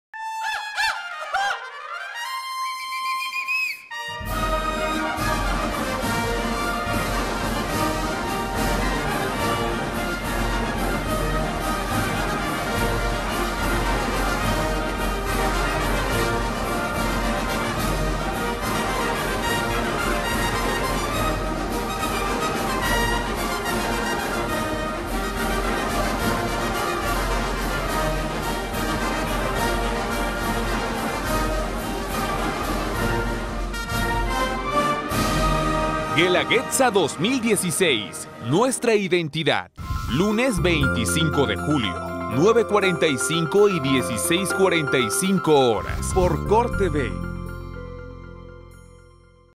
CORTV: Guelaguetza 2016.
Transmisión EN VIVO

Vive NuestraIdentidad a través del color y la tradición de la Guelaguetza 2016.

Lunes 25 de julio 2016
Matutina    09:45 hrs.
Vespertina 16:45 hrs.

Lunes 01 de agosto 2016
Matutina    09:45 hrs.
Vespertina 16:45 hrs.

HD 9.1/Canal 9/ Cable151
Visita nuestra página web: